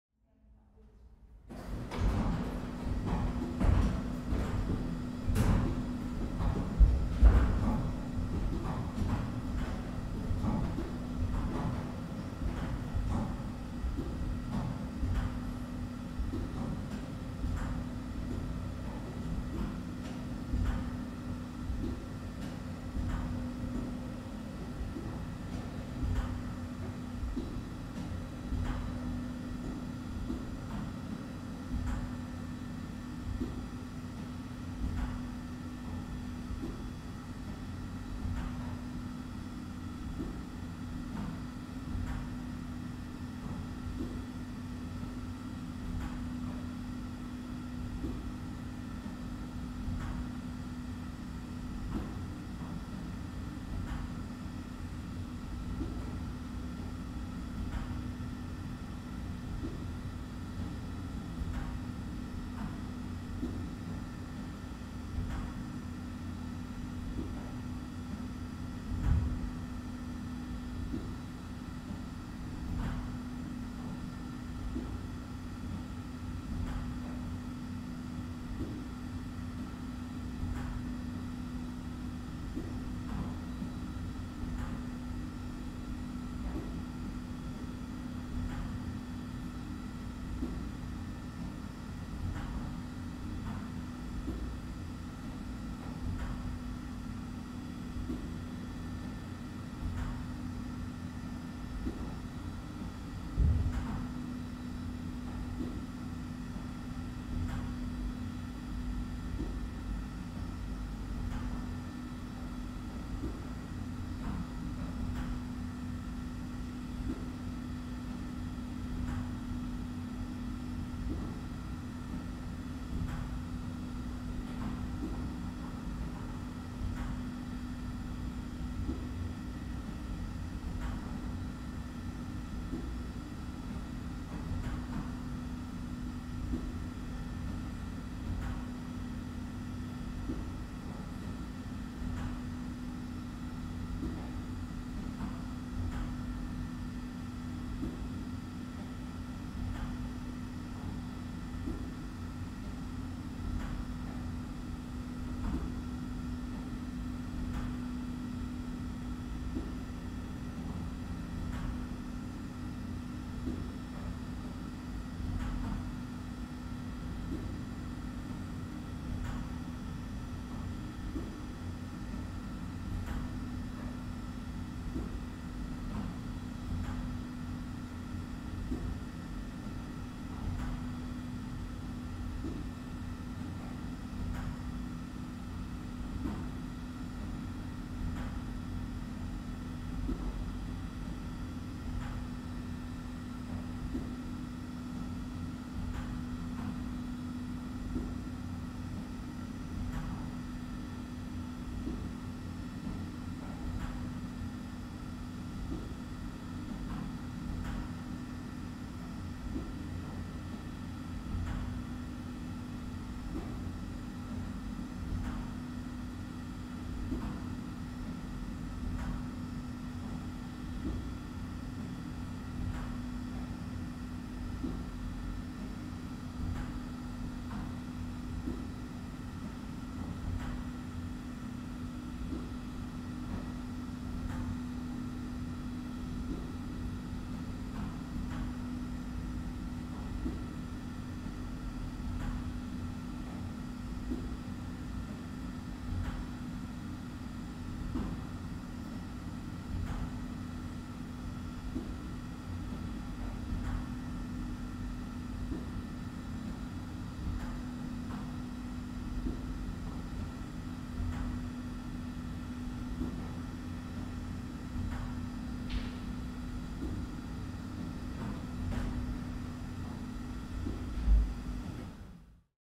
Hydraulics
The organ hydraulics at the Union Chapel Islington, London, UK.